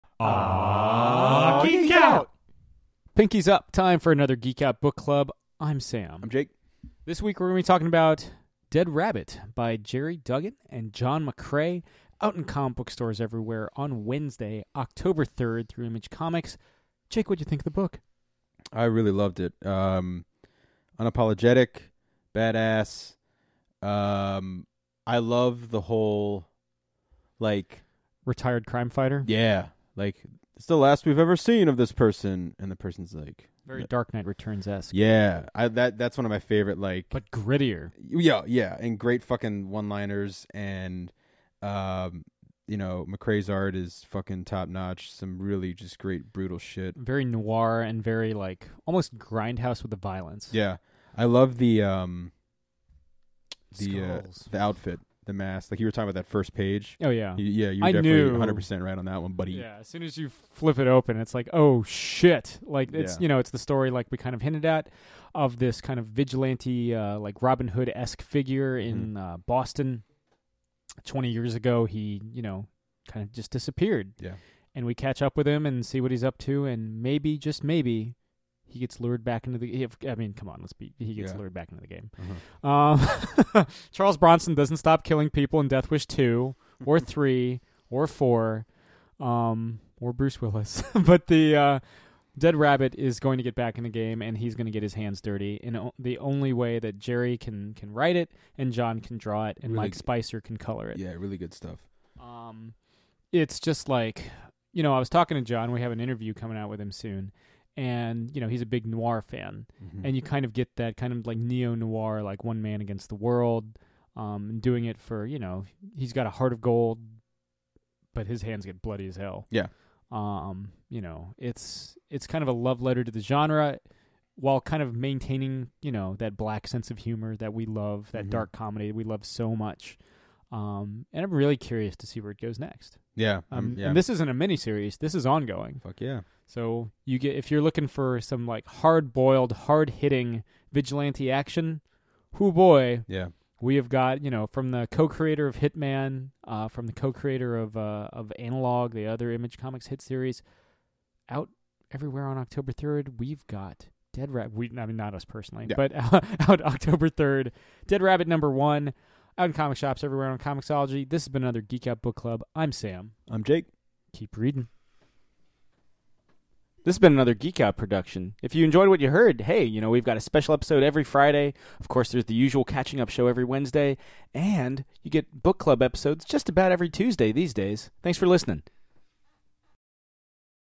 Geek Out Book Club: Dead Rabbit
This week, the boys have an advance review of Dead Rabbit by Gerry Duggan and John McCrea! The new Image Comics series sees a retired Boston-area vigilante return to crimefighting as he deals with his ailing wife. Listen to what Sam and Jake think of the debut issue!

 Dead Rabbit is out in comic shops everywhere and on Comixology on Wednesday, October 3!